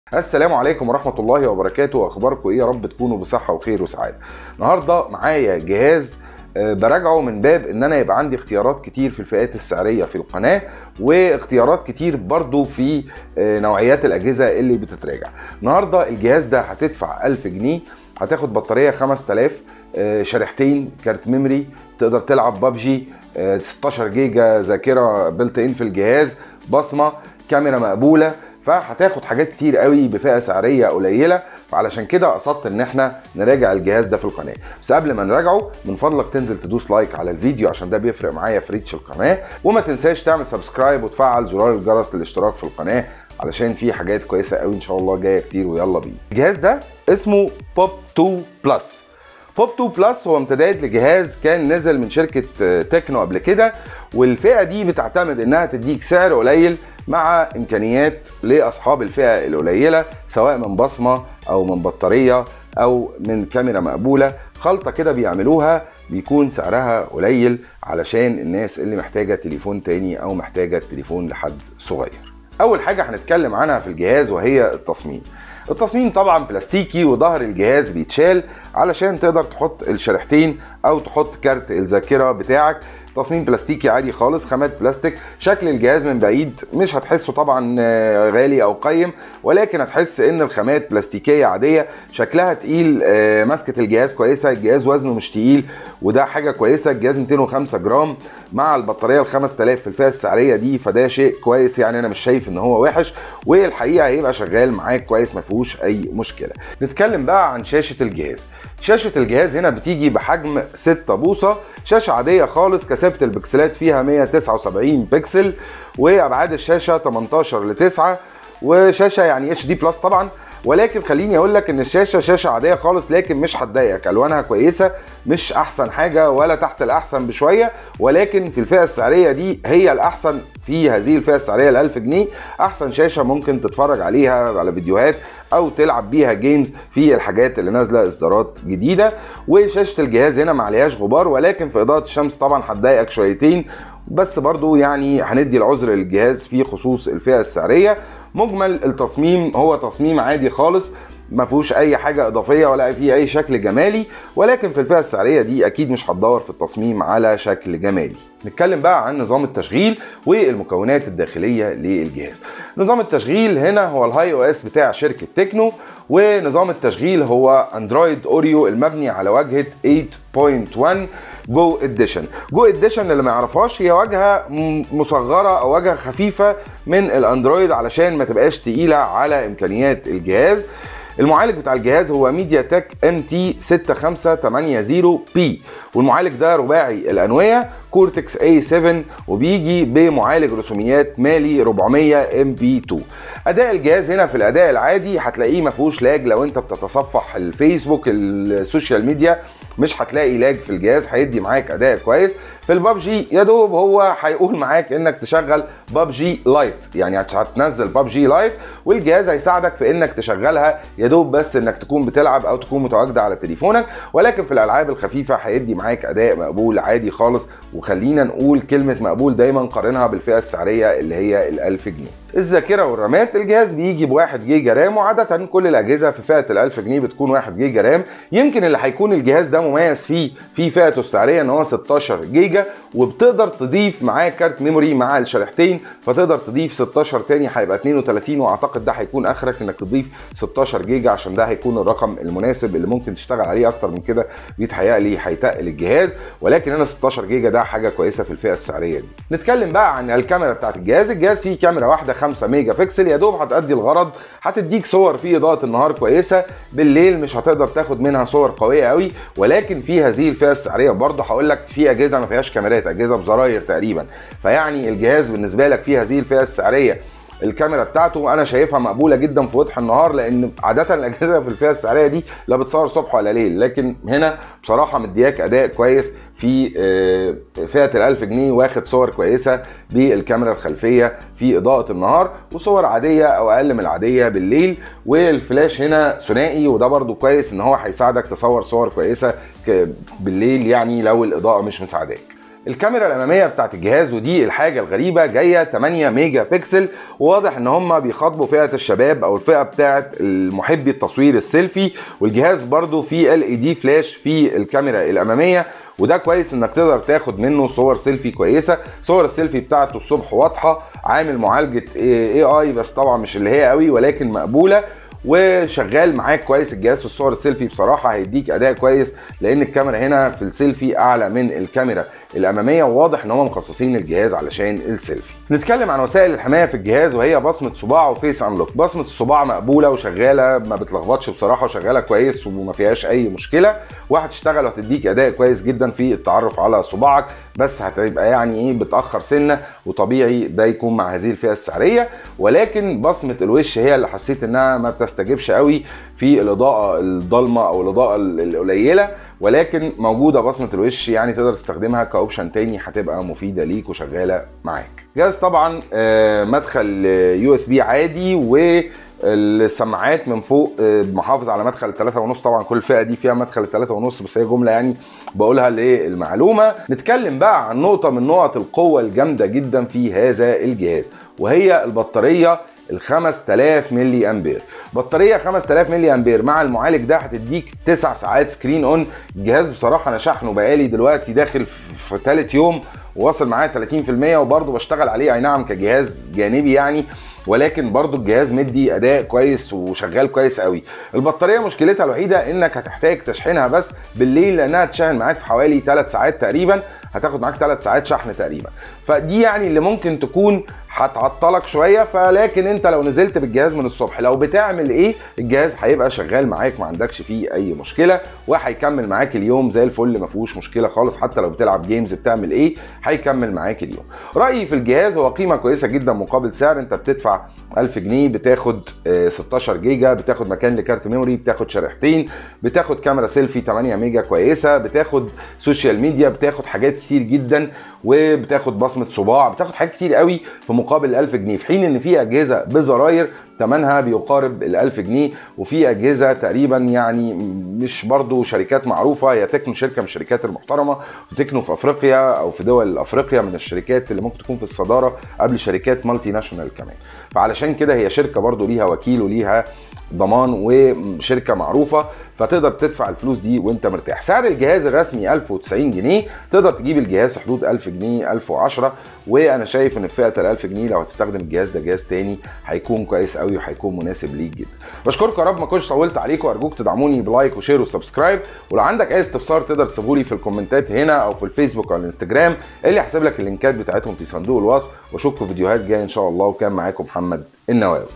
كل ده بألف جنيه | Tecno Pop 2 Plus
هتدفع 1000 جنيه وهتاخد بصمة وش و صباع وبطاريه 5000 وفلاش أمامي وخلفي وتلعب pubg